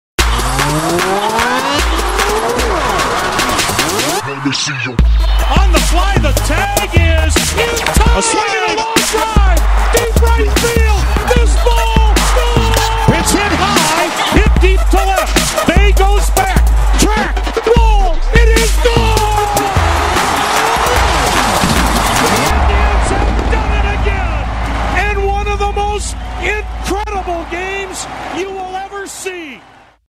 Cleveland Indians "We're Underway" TV commercial
Addy award-winning TV spot for the Cleveland Indians.